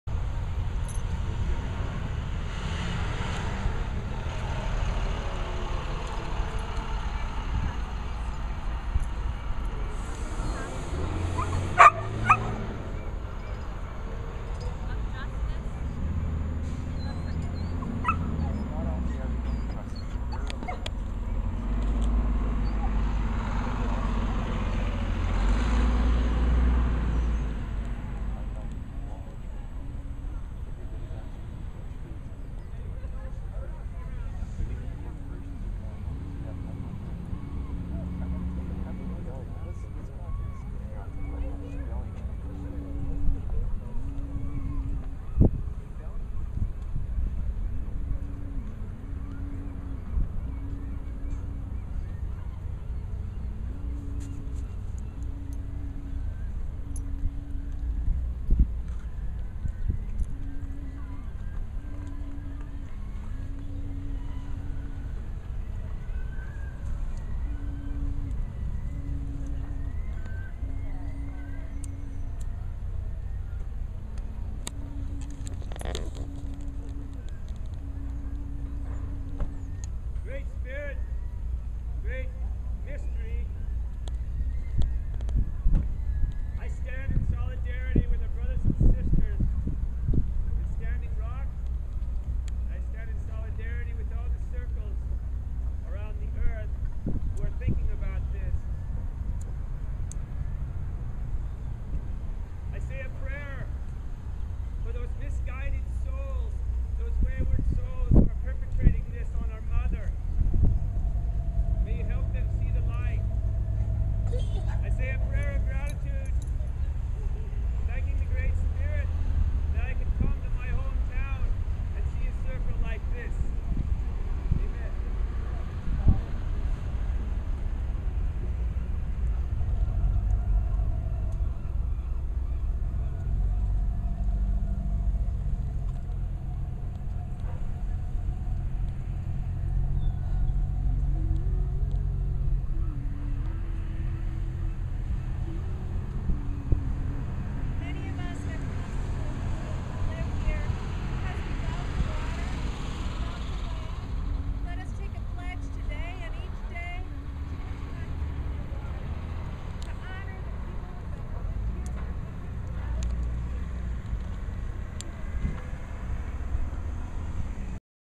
Solidarity with Standing Rock event - Port Townsend WA 09/08/2016
About 90 people gathered and marched in Port Townsend Wa in solidarity with the Standing Rock protesters.  The event organizers stated, "We are offering prayers and good wishes for the September 9th court decision, and continued strength and protection for everyone at the prayer and non violence based Standing Rock Camp."